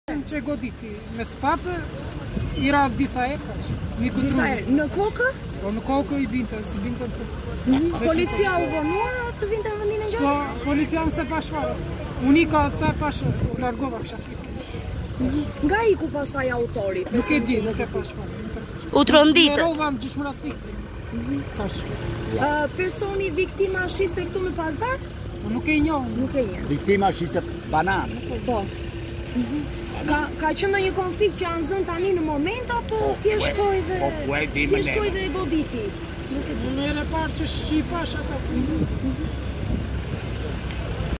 Ora News - Vrasja makabre në Korçë, dëshmitarët tregojnë: E goditi me sëpatë në kokë disa herë
Krimi makabër që ndodhi pak pas mesditës së sotme afër pazarit të qytetit ndodhi në  sy të dhjetra dëshmitarëve.

Viktima është identifikuar si Dashnor Osmani nga fshati Pocestë, i cili punonte si shitës bananesh, ndërkohë personi që e ka goditur me sëpatë është Zamir Topi nga i njëjti fshat.

 Ora News sjell dëshminë e dy prej tyre të cilat tregojnë se janë të shokuar edhe ende nuk e kanë marrë veten.

“E goditi me sëpatë në kokë disa herë. U trondita.  Sa e pashë u largova kisha frikë. U tmerrova desh më ra të fikët” thotë një djalë I cili ka qenë dëshmitar I skenës makabre.

Viktima shiste banane ndërsa autori I cili është arrestuar nuk njihej nga dëshmitarët.

Ngjarja e rëndë ka tronditur qytetin pas ka ndodhur në një zonë të populluar.

Me dhjetëra qytetarë u mblodhën rreth trupit të pajetë ndërsa pritej të zhvendosej në autoambulancë.